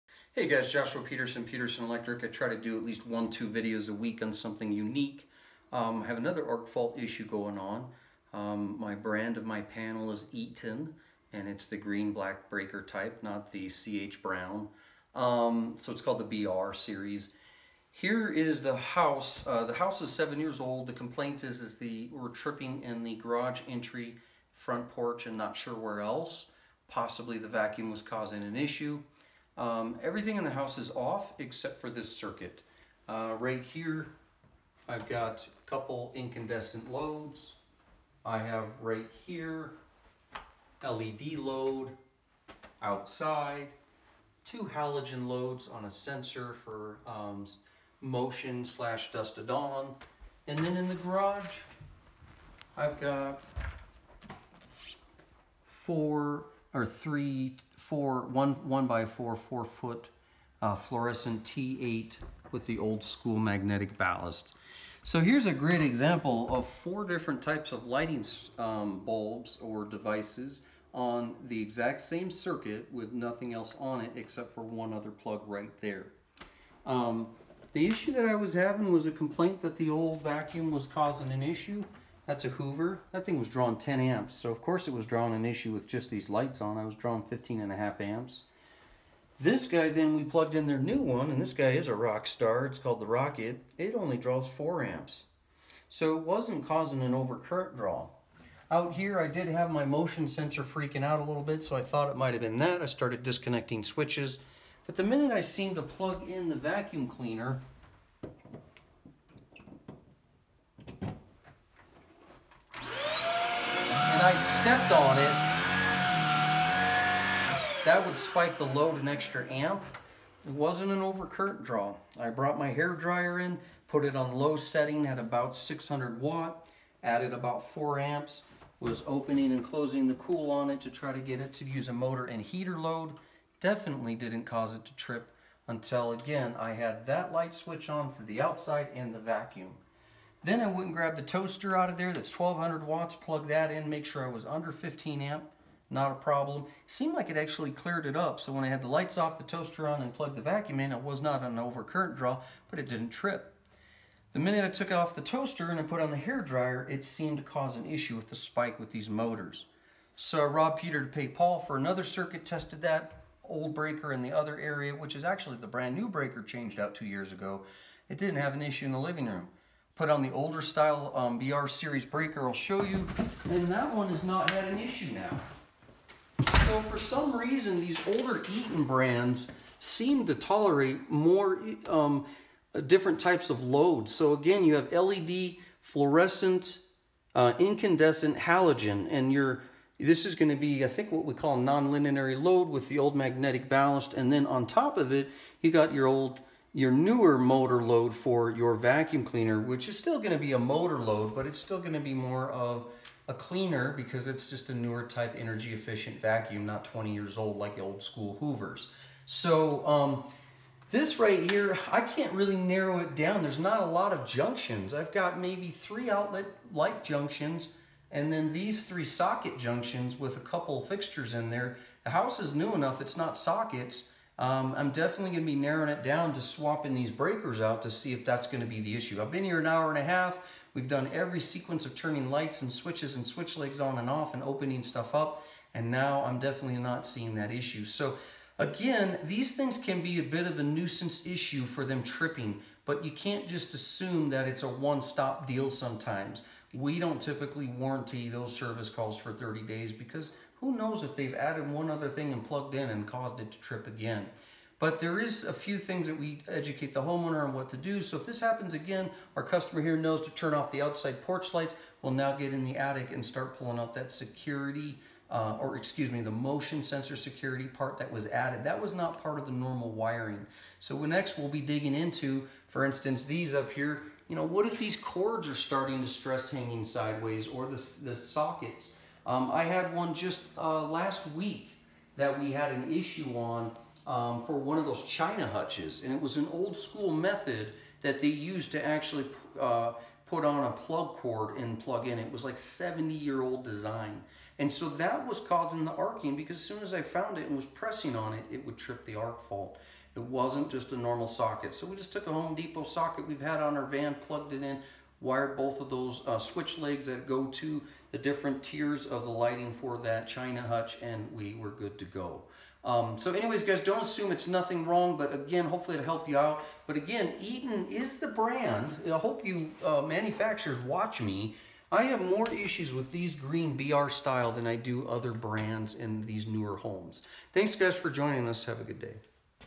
Arc Fault Issue with an Eaton Panel, BR Series
Breakers were tripping in the garage entry and front porch and other areas in a 7-year-old home due to an old vacuum cleaner that drew 10 amps.